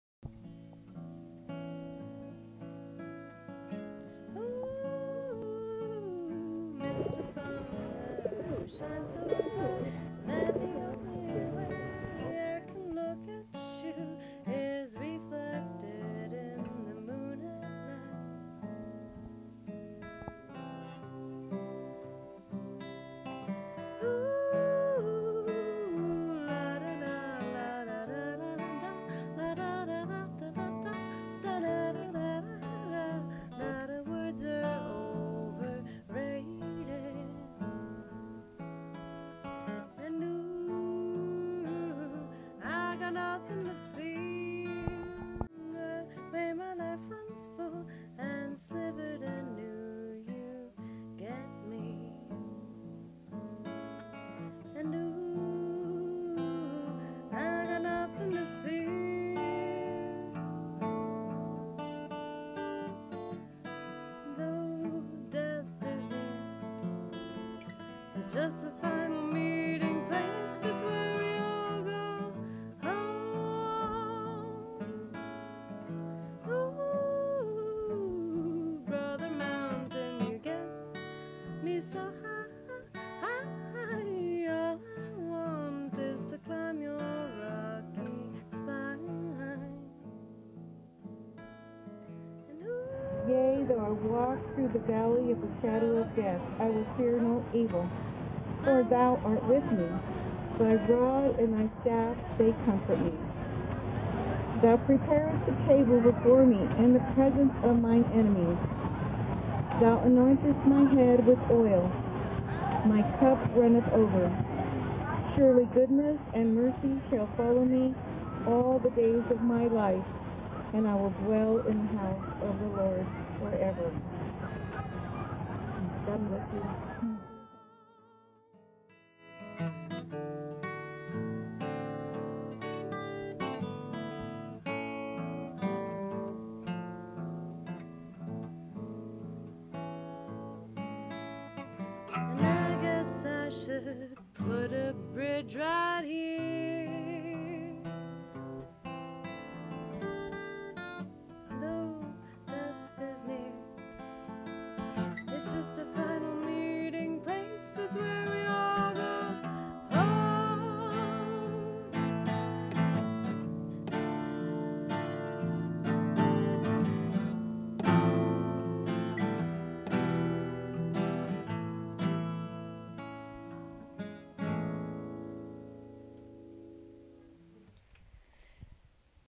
Bob Chavez Memorial Dove Release
Denver Dove Release, Bob Chavez Memorial Service September 1 2012